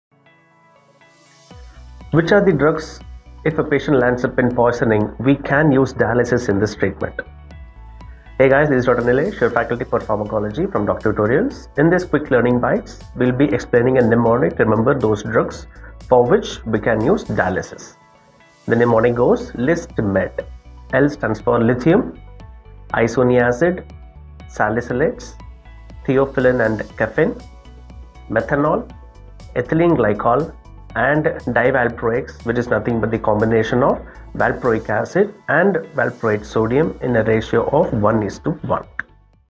DocTutorials' QuickLearningByte | DIALYSIS IN DRUG POISONING- PHARMACOLOGY | Dr. Nileshraj
Watch today's Quick Learning Byte to learn DIALYSIS IN DRUG POISONING from PHARMACOLOGY subject by  Dr. Nileshraj